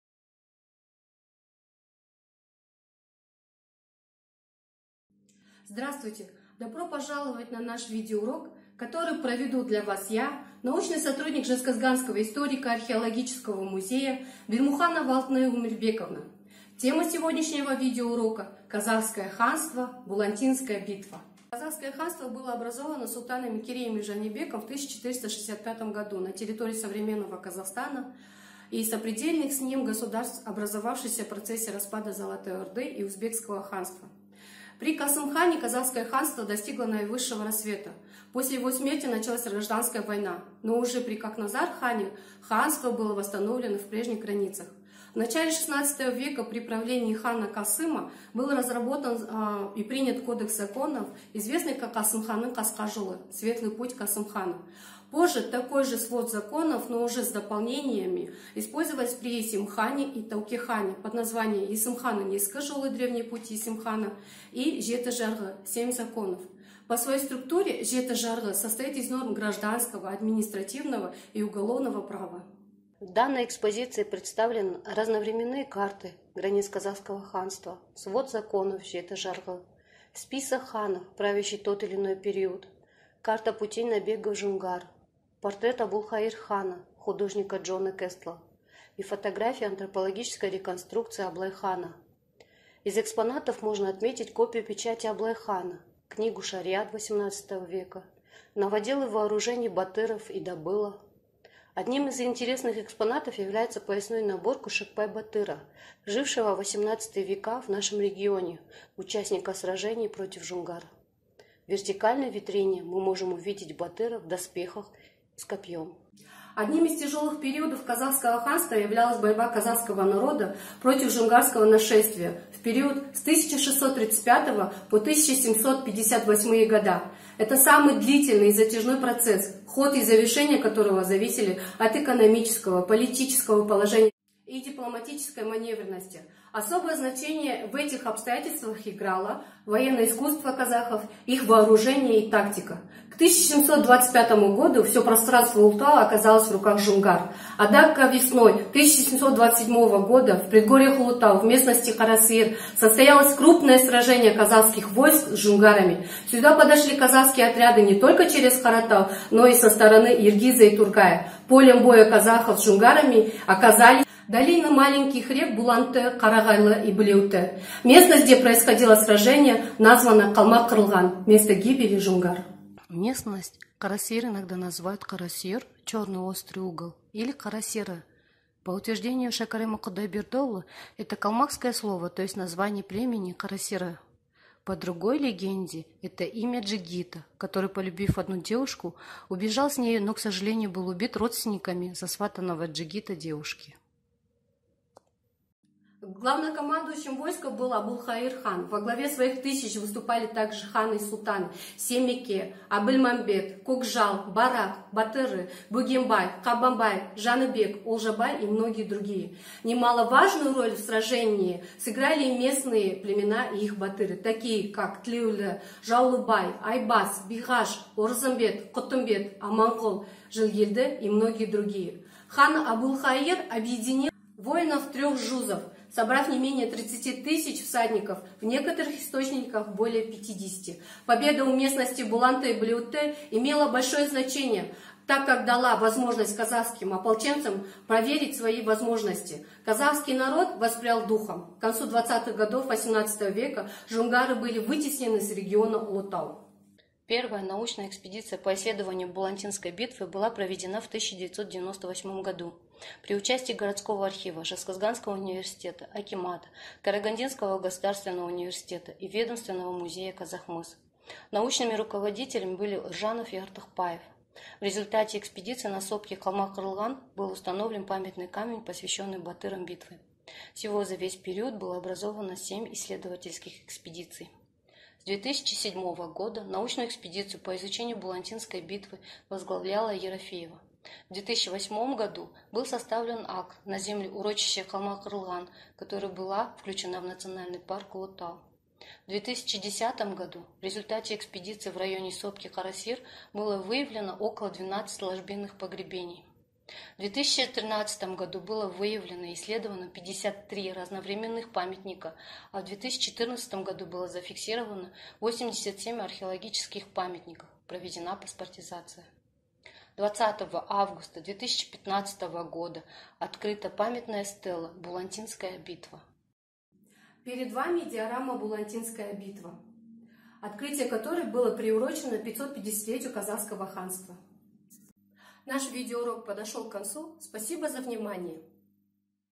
История Казахского ханства: Булантинская битва
История Казахского ханства, сражение в местности «Калмак кырылган», переломная роль Булантинской битвы в борьбе казахского народа против жунгар

Автор видеоурока: КГКП "Жезказганский историко-археологический музей с выставочным залом"